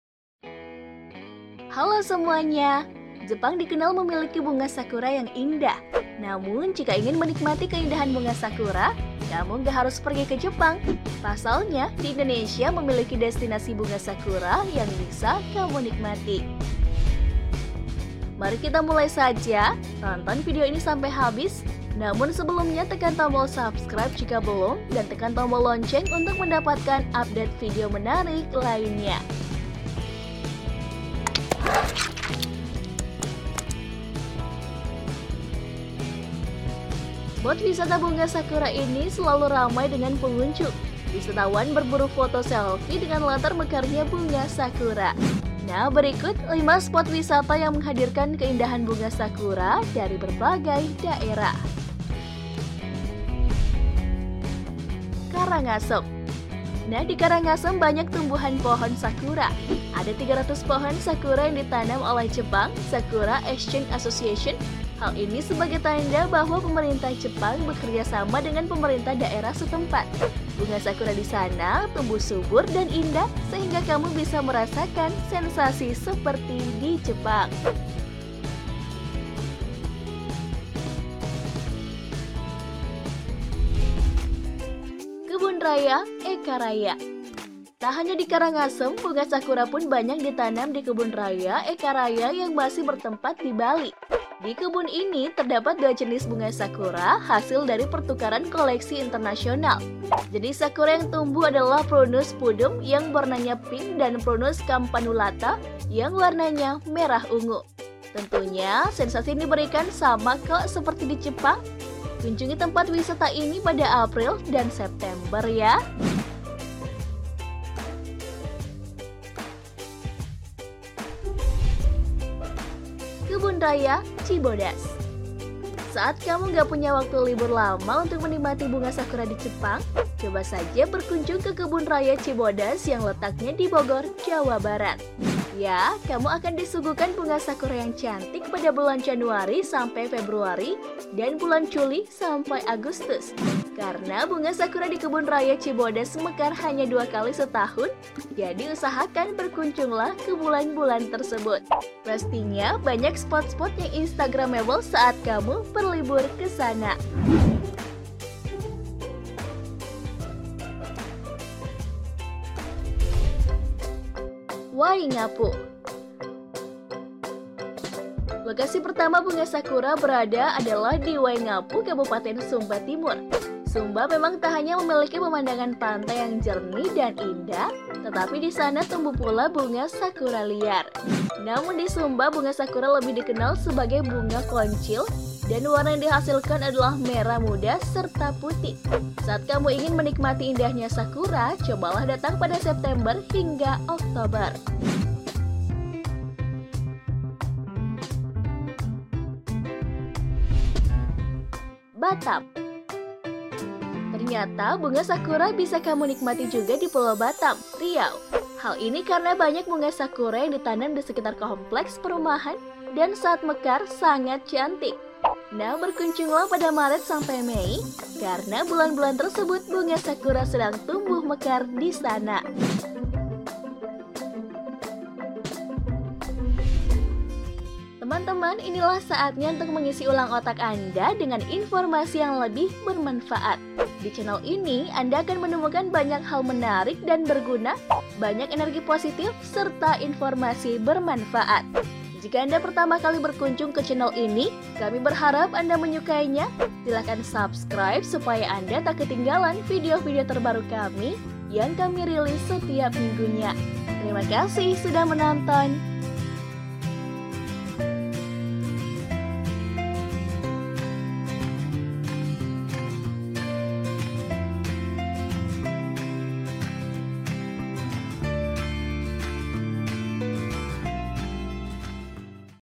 Gak Harus ke Jepang, Ini 5 Spot Wisata Bunga Sakura di Indonesia
Gak Harus ke Jepang, Ini 5 Spot Wisata Bunga Sakura di Indonesia. Jepang dikenal memiliki bunga sakura yang indah. Namun, jika ingin menikmati keindahan bunga sakura, kamu gak harus pergi ke Jepang. Pasalnya Indonesia memiliki destinasi bunga sakura yang bisa kamu nikmati.

Spot wisata bunga sakura ini selalu ramai dengan pengunjung. Wisatawan berburu foto selfie dengan latar mekarnya bunga sakura. Nah berikut 5 spot wisata yang menghadirkan keindahan bunga sakura dari berbagai daerah.

1. Waingapu

Lokasi pertama bunga sakura berada adalah di Wangaipu Kabupaten Sumba Timur. Sumba memang tak hanya memiliki pemandangan pantai yang jernih dan indah tetapi di sana tumbuh pula bunga sakura liar. Namun di Sumba bunga sakura lebih dikenal sebagai bunga konjil dan warna yang dihasilkan adalah merah muda serta putih. Saat kamu ingin menikmati indahnya sakura cobalah datang pada September hingga Oktober.

2. Kebun Raya Cibodas

Saat kamu gak punya waktu libur lama untuk menikmati bunga sakura di Jepang, coba saja berkunjung ke Kebun Raya Cibodas yang letaknya di Bogor, Jawa Barat. Ya, kamu akan disuguhkan bunga sakura yang cantik pada bulan Januari sampai Februari dan Bulan Juli sampai Agustus. Karena bunga sakura di Kebun Raya Cibodas mekar hanya dua kali setahun usahakan berkunjunglah ke bulan-bulan tersebut. Pastinya banyak spot-spot yang instagramable saat kamu berlibur kesana.

3. Batam

Ternyata bunga sakura pun bisa kamu nikmati di Pulau Batam, Riau. Hal ini karena banyak bunga sakura yang ditanam di sekitar kompleks perumahan dan saat mekar sangat cantik. Nah berkunjunglah pada Maret sampai Mei karena bulan-bulan tersebut bunga sakura sedang tumbuh mekar di sana.

4. Karangasem

Nah di Karangasem banyak tumbuh pohon sakura. Ada 300 pohon sakura yang ditanam oleh Japan Sakura Exchange Association. Hal ini sebagai tanda bahwa Pemerintah Jepang bekerjasama dengan pemerintah daerah setempat. Bunga sakura di sana tumbuh subur dan indah sehingga kamu bisa merasakan sensasi seperti di Jepang.

5. Kebun Raya Eka Karya

Tak hanya di Karangasem bunga sakura pun banyak ditanam di Kebun Raya Eka Raya yang masih bertempat di Bali. Di kebun ini terdapat dua jenis bunga sakura hasil dari pertukaran koleksi internasional. Jenis sakura yang tumbuh adalah Prunus puddum yang warnanya pink dan Prunus campanulata yang warnanya merah ungu. Tentunya sensasi yang diberikan sama kok seperti di Jepang. Kunjungi tempat wisata ini pada April dan September ya!

Artikel by : Indah Shaliha idntimes